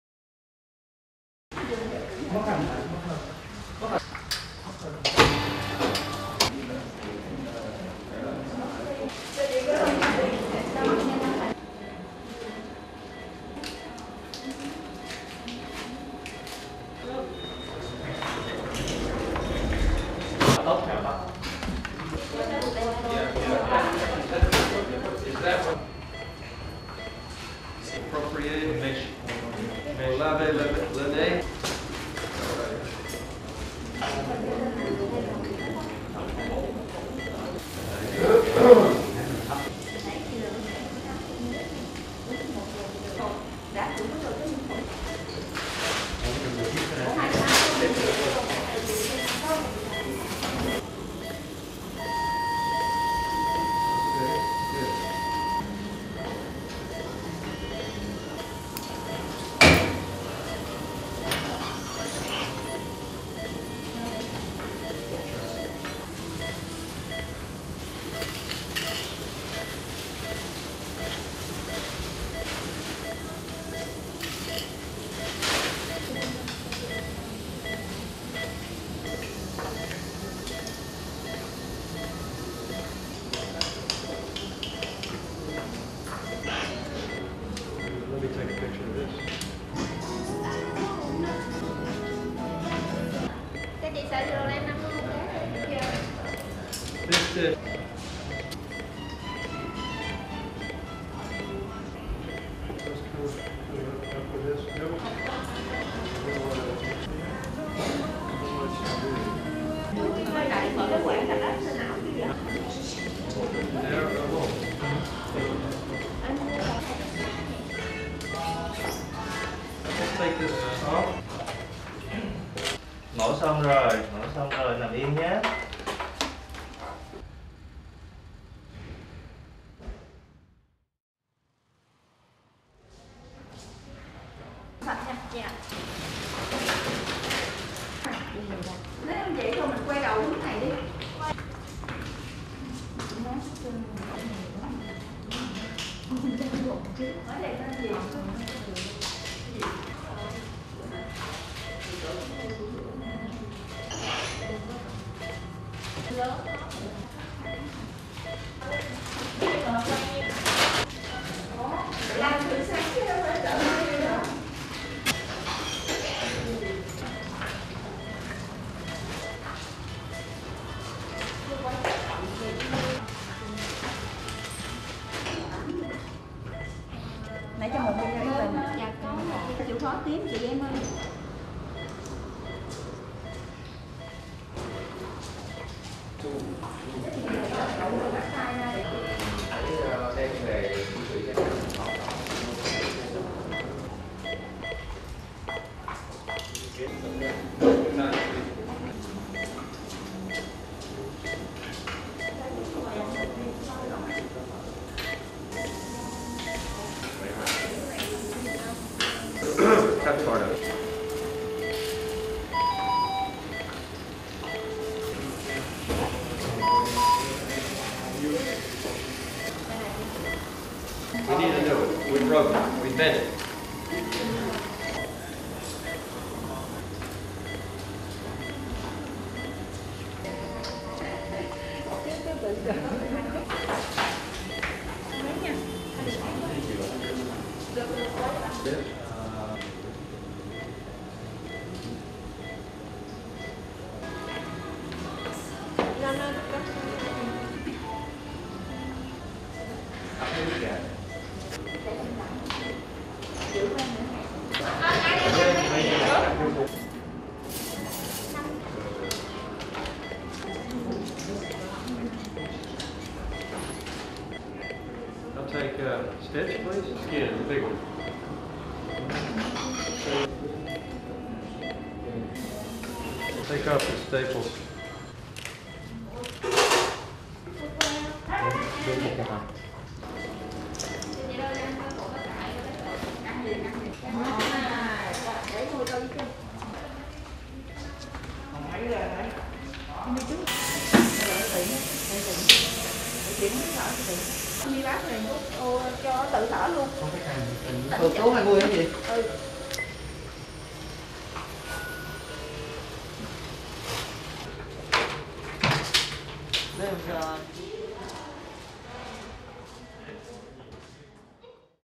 Clip tổng hợp diễn biến ca phẫu thuật của Nguyễn Hoàng Thiên Ân và Lê Hoàng Em
Bác sĩ McKay McKinnon (đến từ Mỹ) và ê-kíp bác sĩ tại Bệnh viện FV đã phẫu thuật thành công loại bỏ khối u vùng mặt gây biến dạng hoàn toàn khuôn mặt cho hai bệnh nhân Nguyễn Hoàng Thiên Ân (18 tuổi, ngụ tỉnh Bà Rịa Vũng Tàu) và Lê Hoàng Em (31 tuổi, ngụ tỉnh Đồng Tháp).

Bệnh nhân Nguyễn Hoàng Thiên Ân được mổ vào lúc 10 giờ sáng ngày 30/07/2013

Bệnh nhân Lê Hoàng Em được mổ vào ngày 31/07/2013